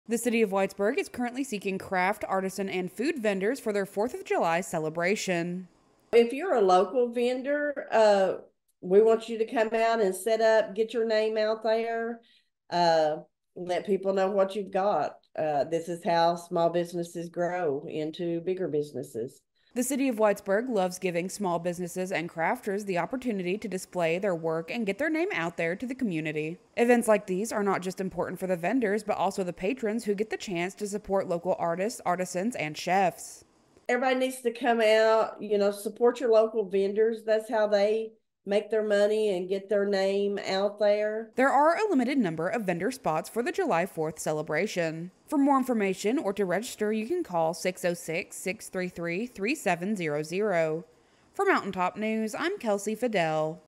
Whitesburg is Looking for Fourth of July Vendors
The City of Whitesburg is looking for vendors for its Fourth of July celebration.